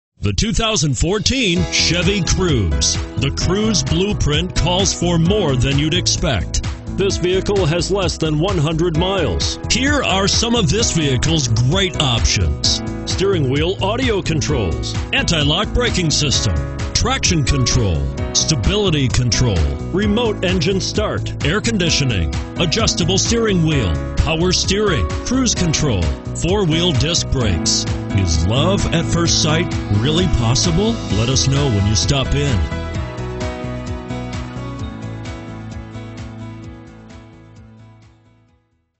2014 CHEVROLET CRUZE White Bear Lake, MN 44781
2014 CHEVROLET CRUZE White Bear Lake, MN
Stock# 44781

1801 E Co Road F

You'll love this 2014 Chevrolet Cruze. This is a car you'll want to take home. With 100 miles, it features Automatic transmission and an exterior color of Atlantis Blue Metallic. Call us and be the first to open the car door today!